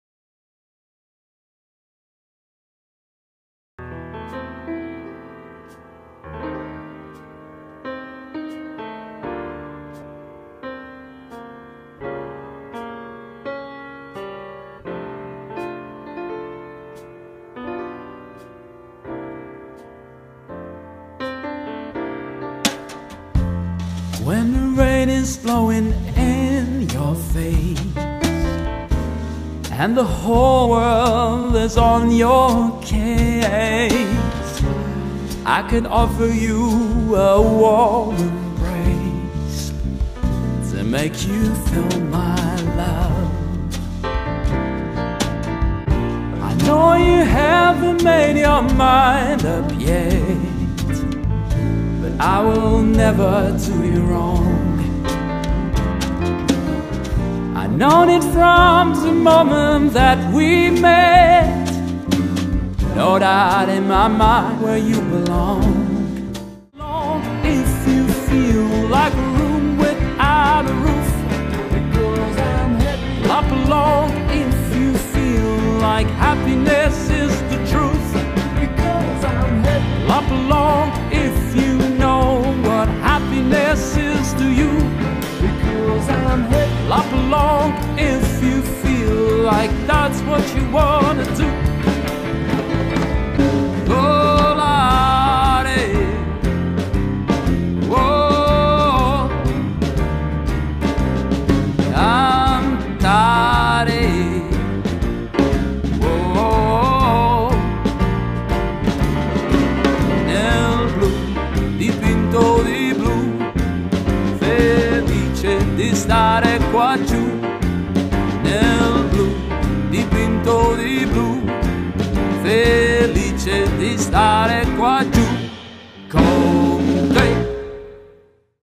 Speakeasy Fellas - AliveNetwork.com
Speakeasy Fellas are a 5 piece live band performing all time classics with a Latin, Jazz & Postmodern Jukebox inspired twist! Perfect for every mood and event, from lounge entertainment to dance floor filling parties, this exciting band give Bruno Mars the swing treatment, George Michael a hit of Manouche Jazz & more!